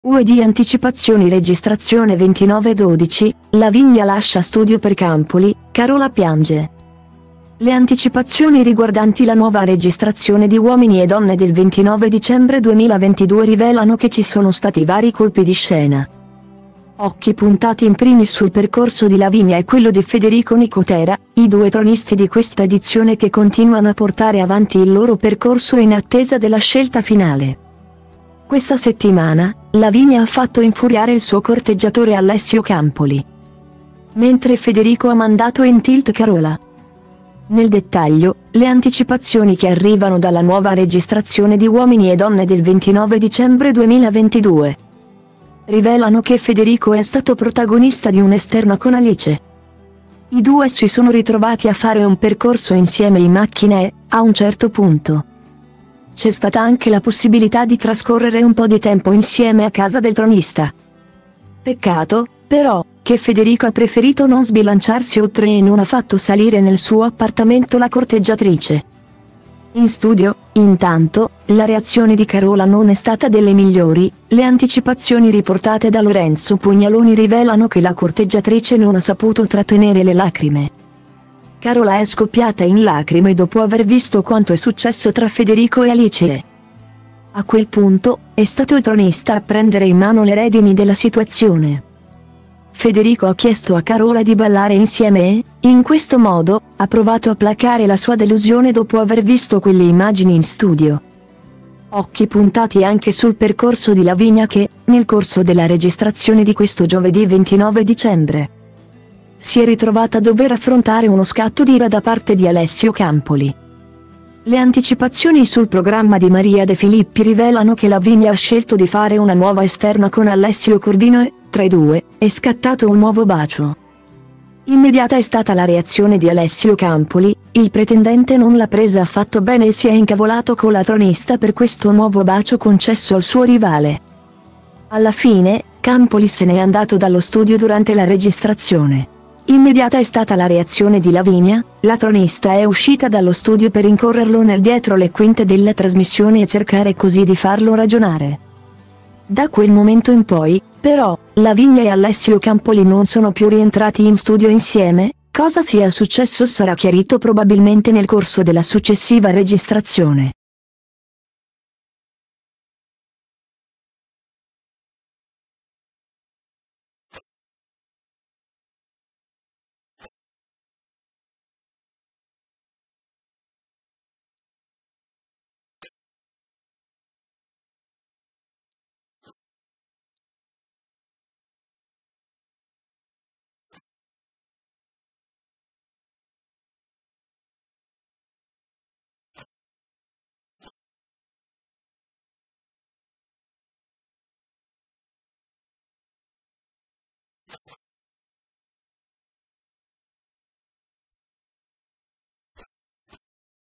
Uomini e Donne anticipazioni registrazione 29/12: Lavinia lascia studio per Campoli, Carola piange
Le anticipazioni riguardanti la nuova registrazione di Uomini e donne del 29 dicembre 2022 rivelano che ci sono stati vari colpi di scena. Occhi puntati in primis sul percorso di Lavinia e quello di Federico Nicotera, i due tronisti di questa edizione che continuano a portare avanti il loro percorso in attesa della scelta finale. Questa settimana, Lavinia ha fatto infuriare il suo corteggiatore Alessio Campoli,